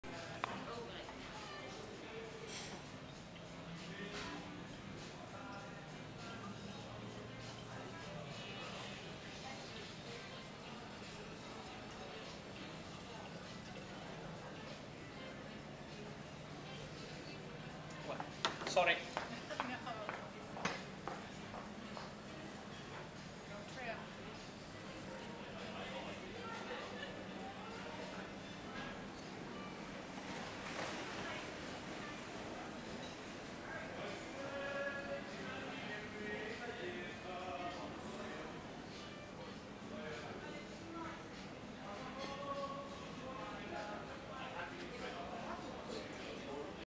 patio dinning at il Buco, Sorrento, Italy - October 2008
patio dinning at il Buco, Sorrento, Italy. This restaurant was recommended by Frommer's Travel Guide and Rick Steves Travel Europe